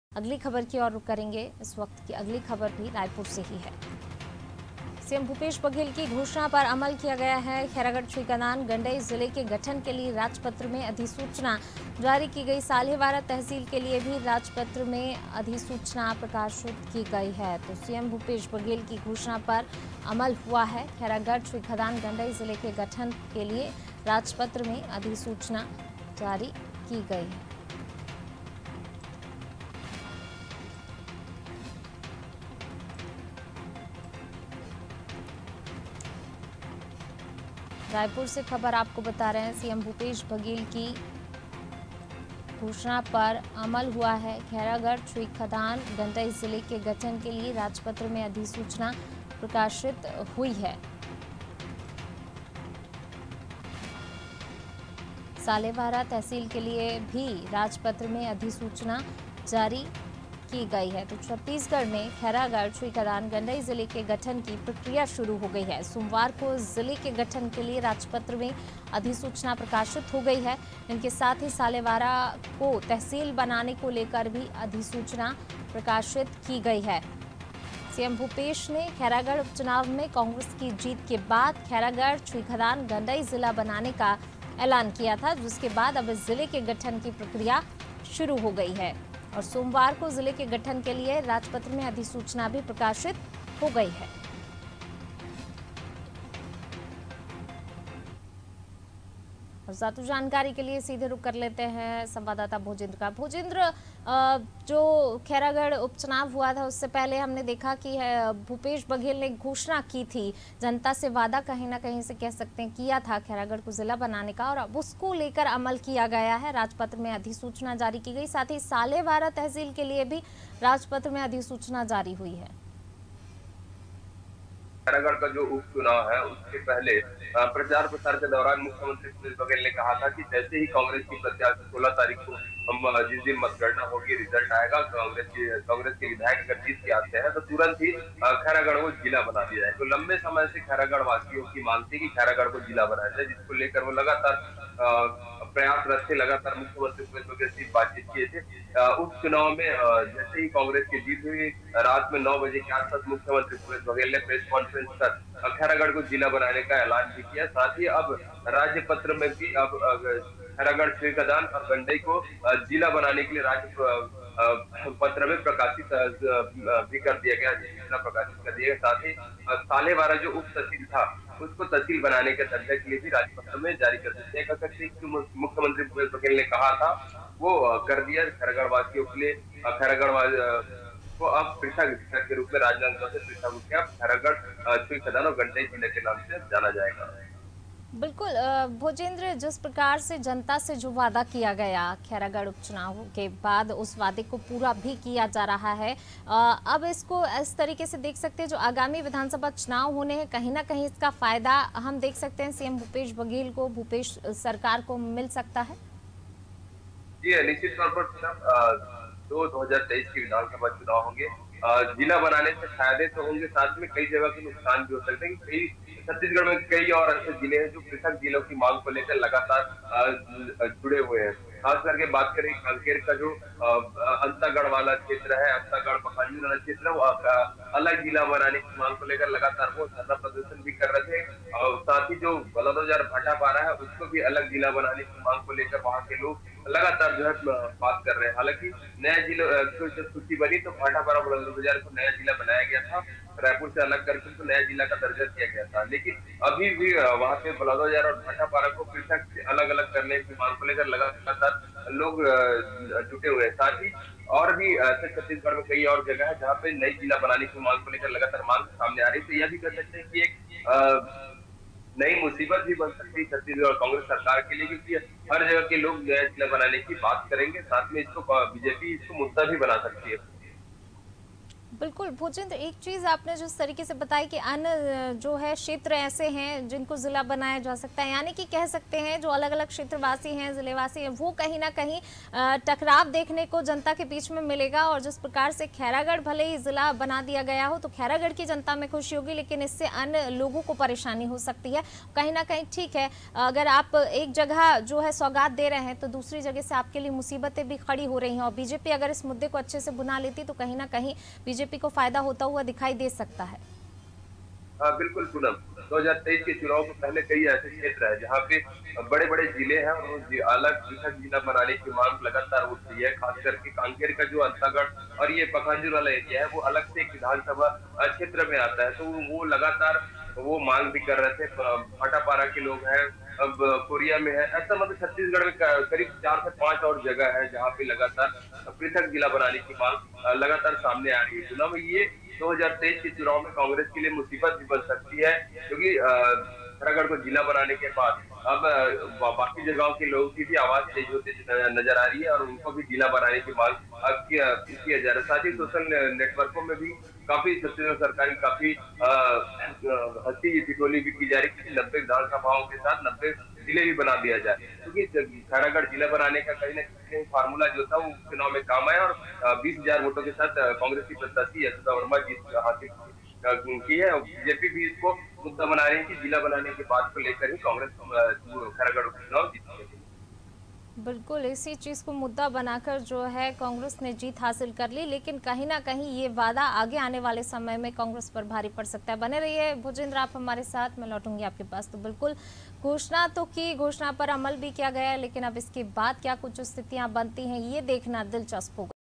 Raipur : सीएम भूपेश बघेल की घोषणा पर अमल | CG News Hindi
रायपुर सीएम भूपेश बघेल की घोषणा पर अमल खैरागढ़-छुईखदान-गंडई जिले के गठन की अधिसूचना जिले के गठन के लिए राजपत्र में अधिसूचना प्रकाशित सीएम भूपेश बघेल की घोषणा पर अमल

Shivraj Singh Chouhan
Bhupesh Baghel
mp cg news live
Chhattisgarh News
Jyotiraditya Scindia